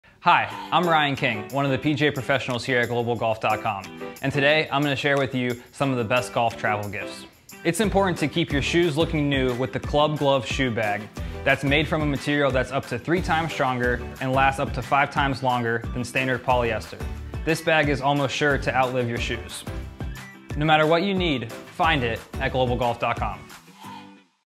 Traveling Golfer Club Glove Shoe Bag
Shoes like to travel too & there is no better way to treat your shoes than with the Club Glove shoe bag,